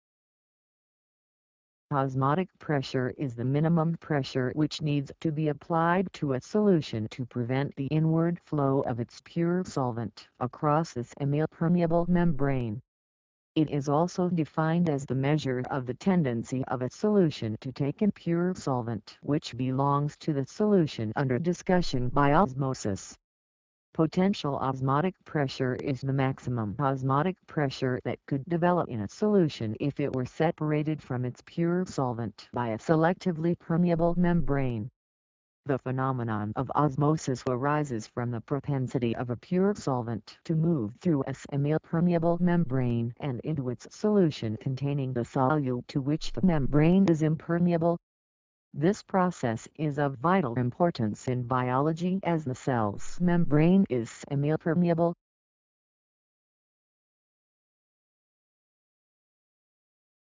What Is OSMOTIC PRESSURE? OSMOTIC PRESSURE Definition & Meaning
What is OSMOTIC PRESSURE, What does OSMOTIC PRESSURE mean, OSMOTIC PRESSURE meaning, OSMOTIC PRESSURE definition, OSMOTIC PRESSURE explanation

Osmotic pressure is the minimum pressure which needs to be applied to a solution to prevent the inward flow of its pure solvent across a semipermeable membrane.[1] It is also defined as the measure of the tendency of a solution to take in pure solvent (which belongs to the solution under discussion) by osmosis. Potential osmotic pressure is the maximum osmotic pressure that could develop in a solution if it were separated from its pure solvent by a selectively permeable membrane. The phenomenon of osmosis arises from the propensity of a pure solvent to move through a semipermeable membrane and into its solution containing a solute to which the membrane is impermeable. This process is of vital importance in biology as the cell's membrane is semipermeable.
Source: Wikipedia.org